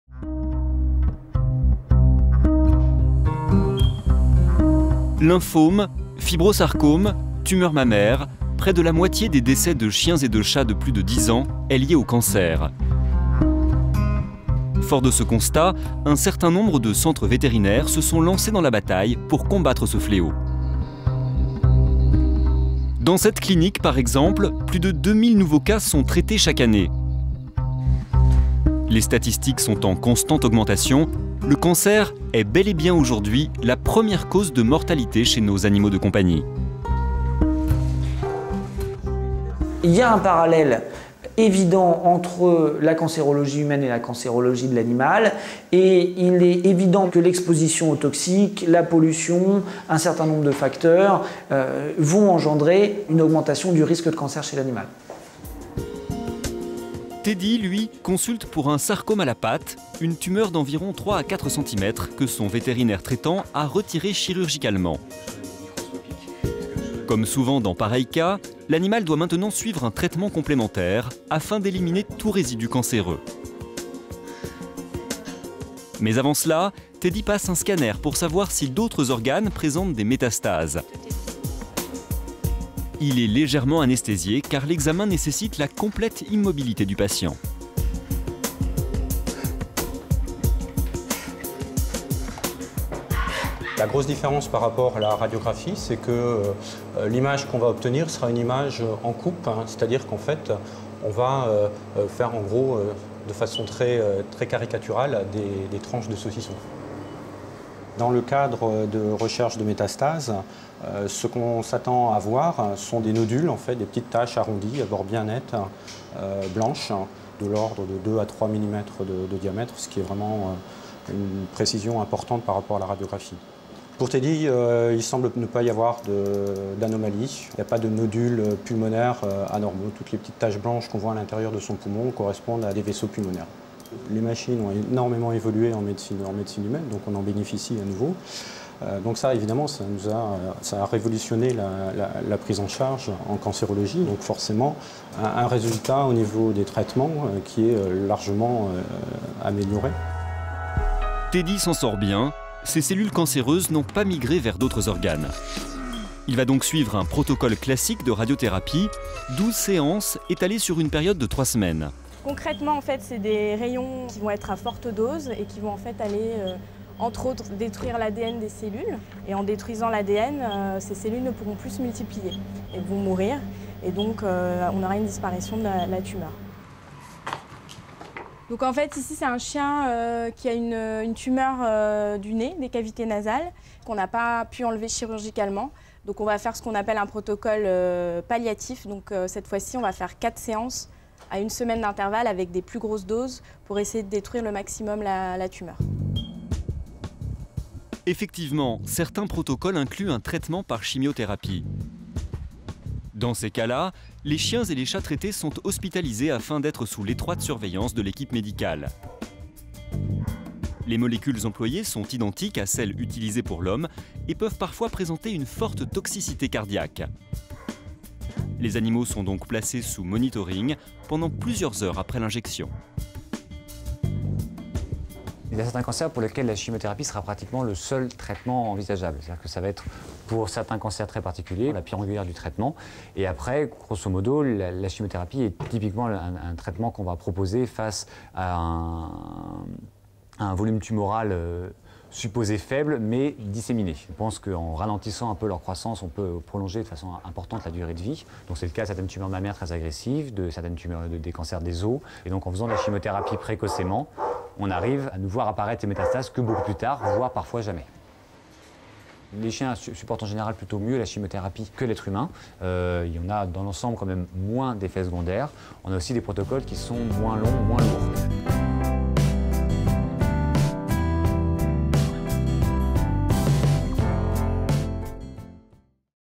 Cancer chez le chien : des traitements existent !
Quels sont les signes du cancer ? Comment le traiter ? Quelles sont les chances de survie ? Quelle est l'espérance de vie des chiens atteints de cancers ? 30 Millions d'Amis fait le point.